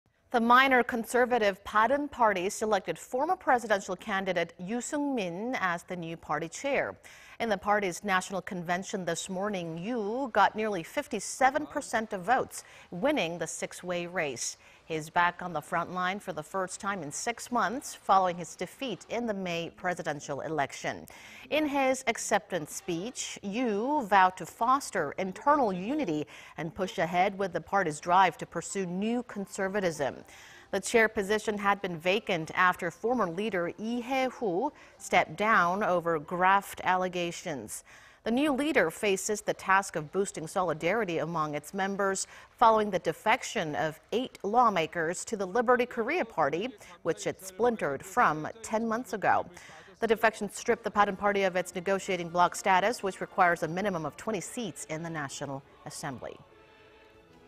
Bareun Party picks former presidential candidate as new leader
바른정당 새 대표에 유승민…"죽음의 계곡서 당 지키겠다"
  The minor conservative Bareun Party selected former presidential candidate Yoo Seung-min as the new party chair.
  In the party's national convention on Monday,... Yoo got nearly 57 percent of votes,... winning the six-way race.
He is back on the frontline for the first time in six months,... following his defeat in the May presidential election.
In his acceptance speech,... Yoo vowed to foster internal unity and push ahead with the party's pursuit of a new conservatism.
The chair position had been vacant after former leader Lee Hye-hoo stepped down over graft allegations.
The new leader faces the task of boosting solidarity among its members,... following the defection of eight lawmakers to the Liberty Korea Party,... which the Bareun party splintered from ten months ago.
The defection stripped the Bareun Party of its negotiating bloc status,... which requires a minimum of 20 seats in the National Assembly.